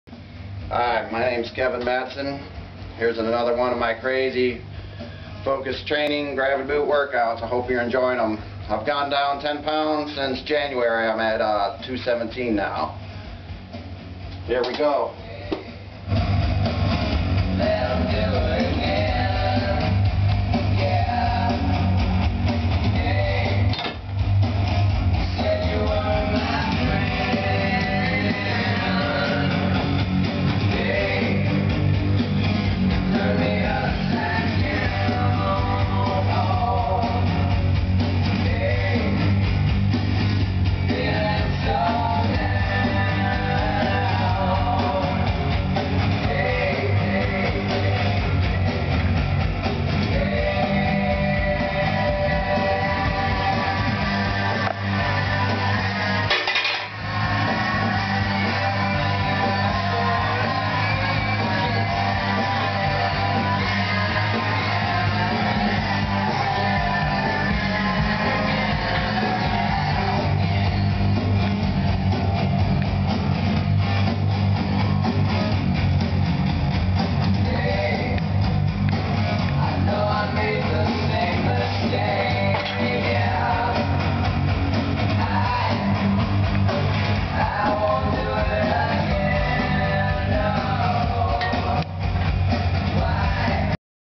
Focused Training Gravity Boot Workout VI
March 26, 2009
Gravity dumbell press ups, & stretch.
I have went down 10 punds in weight since January, weighing in here @ 217.